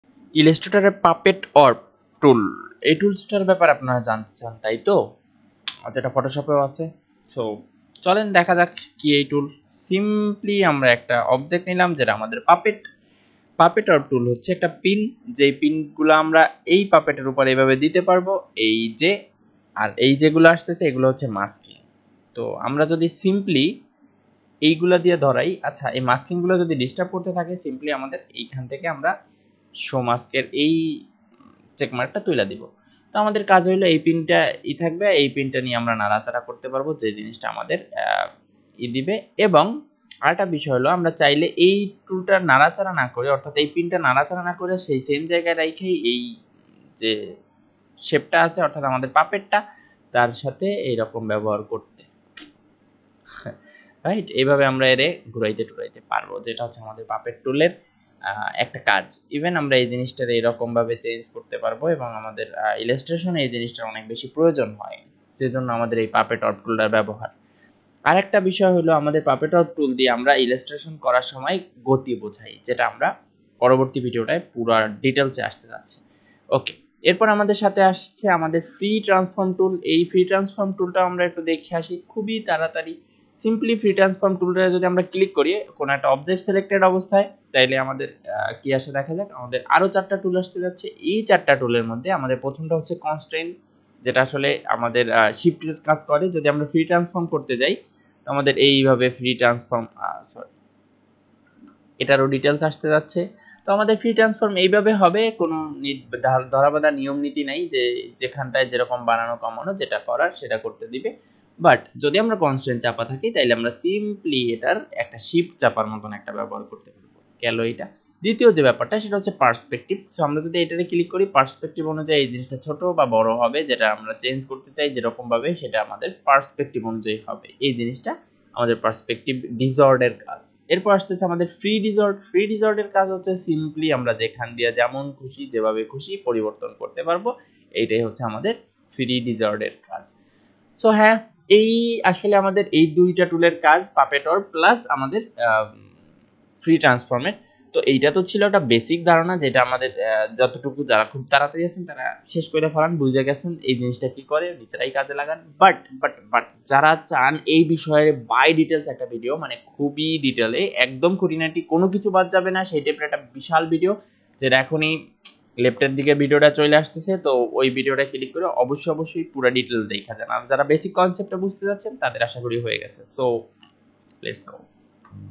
Puppet Warp & Free Transform tool in Adobe Illustrator Bangla tutorial.
In this video, we've discussed the Puppet Warp and Free Transform tool of Adobe illustrator in Bangla.